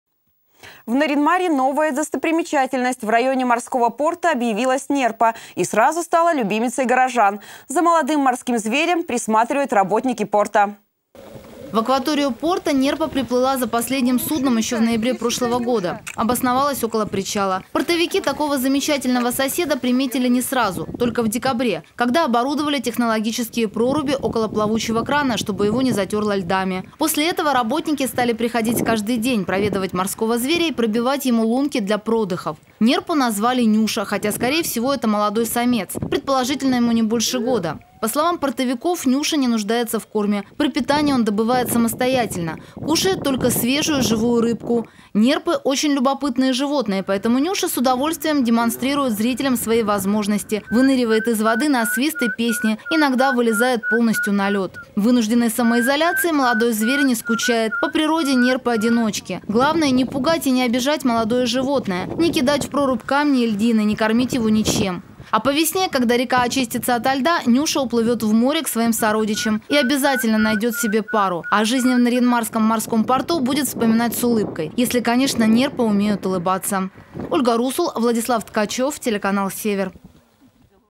ЗНАКОМЬТЕСЬ - НЮША
В Нарьян-Маре новая достопримечательность. В районе морского порта объявилась нерпа. И сразу стала любимицей горожан. За молодым морским зверем присматривают работники порта.